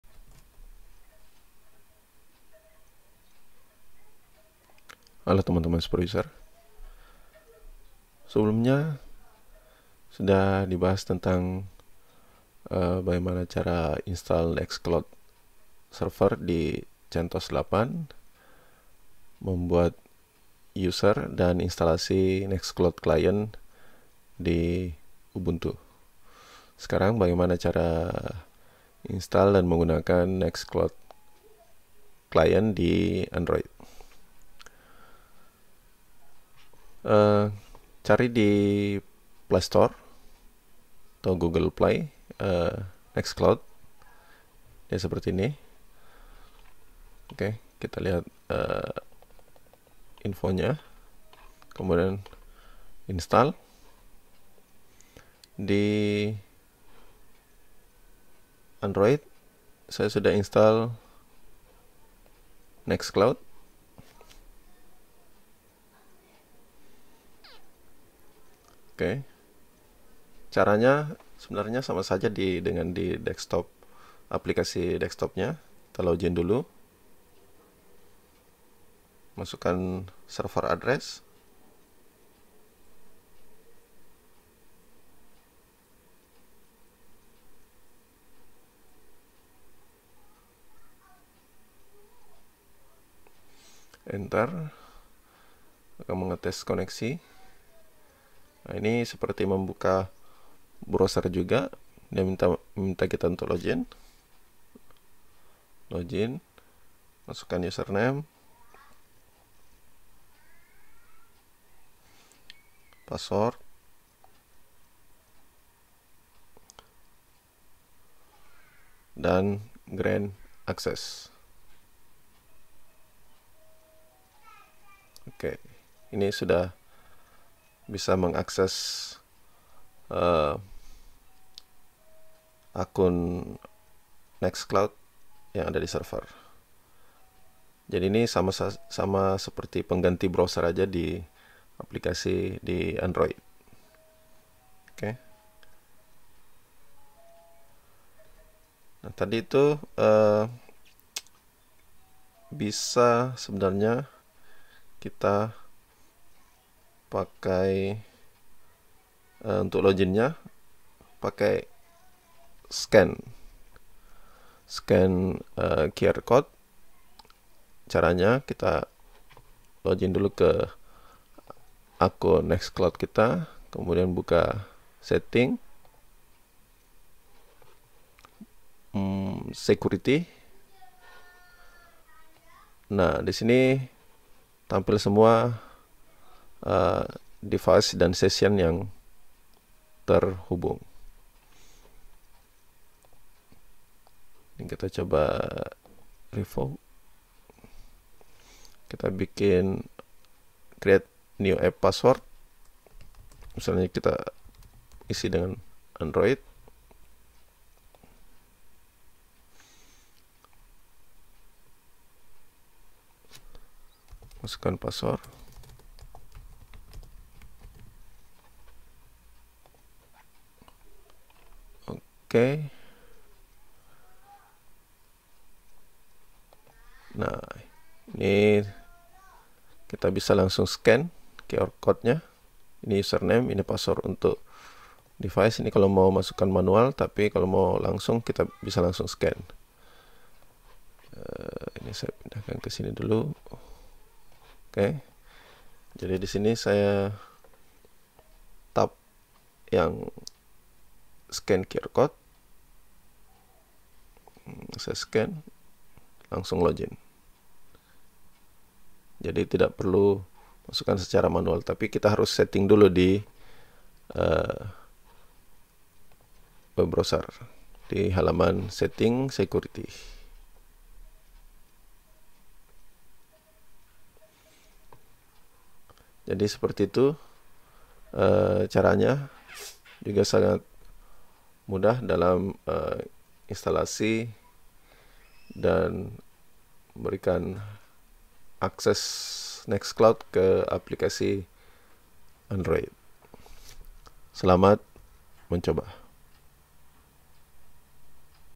Install Nextcloud Client di Android
Bagaimana cara install aplikasi Nextcloud client di Android.